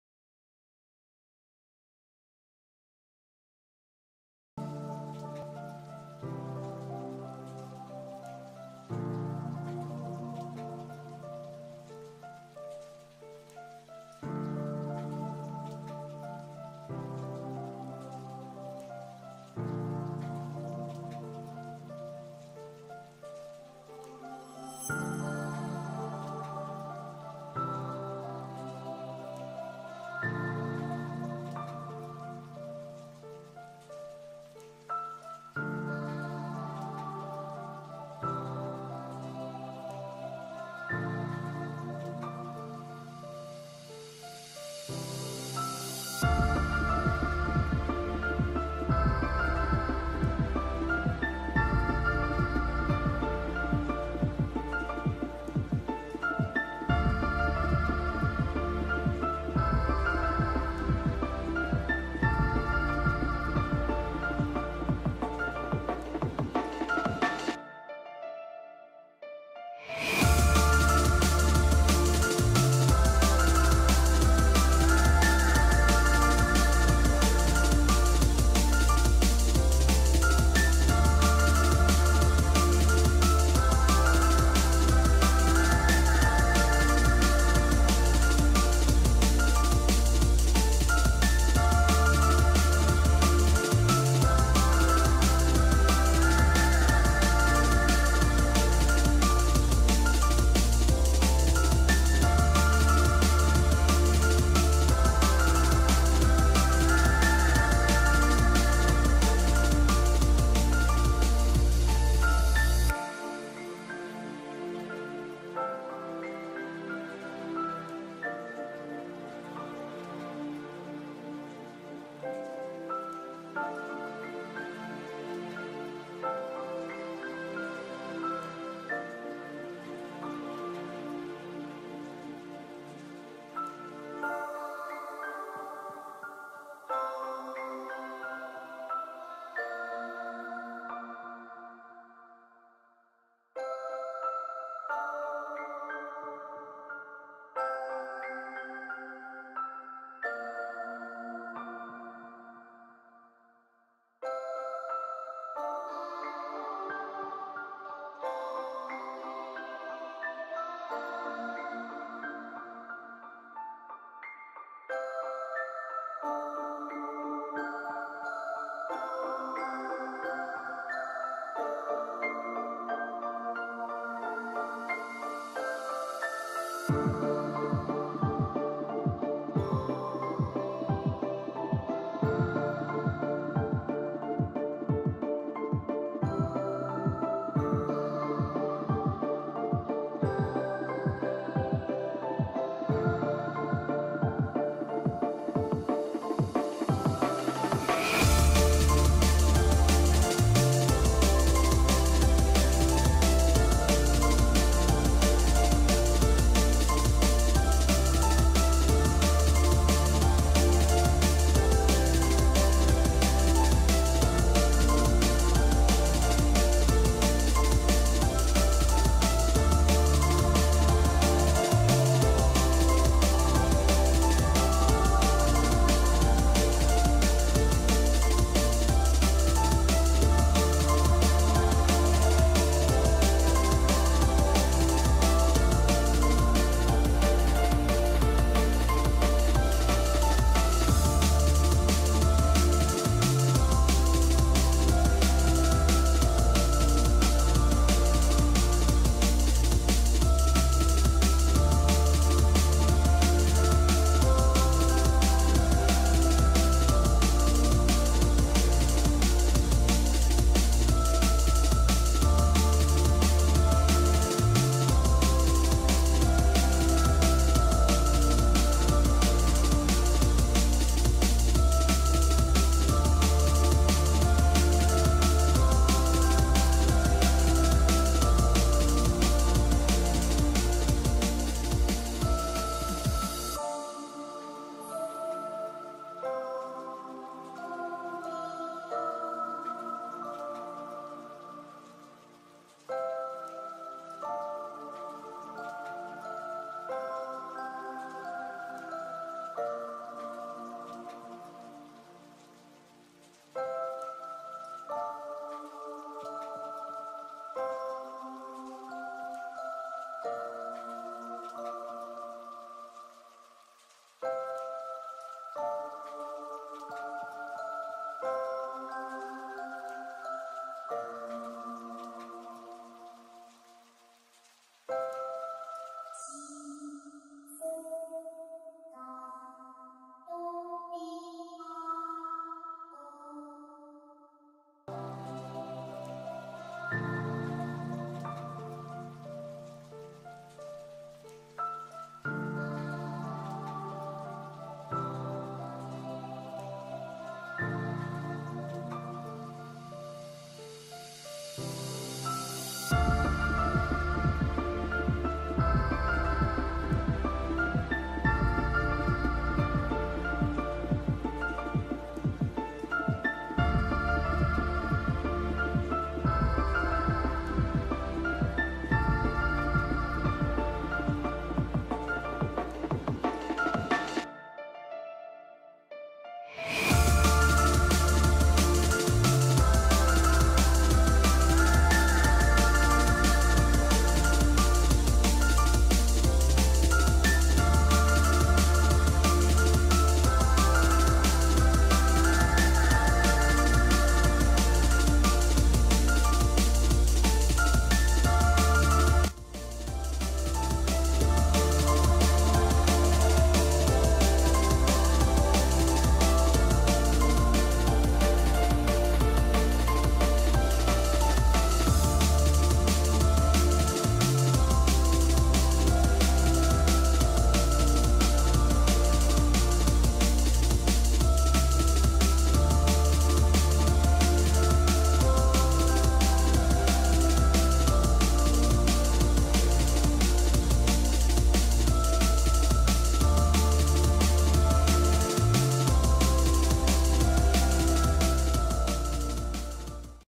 SPEEDPAINT: DVA Lunar New Year | Overwatch 「Paint Tool Sai 」
This time I bring you a little baby D.Va in her Lunar New Year skin!
I did this artwork on stream and with a strong crayon texture, to give it this childish baby D.Va feeling!~

• OS: Windows 10
• Screen Recorder for speedpaints: Open Broadcaster
• Tablet: Wacom Bamboo Pen & Touch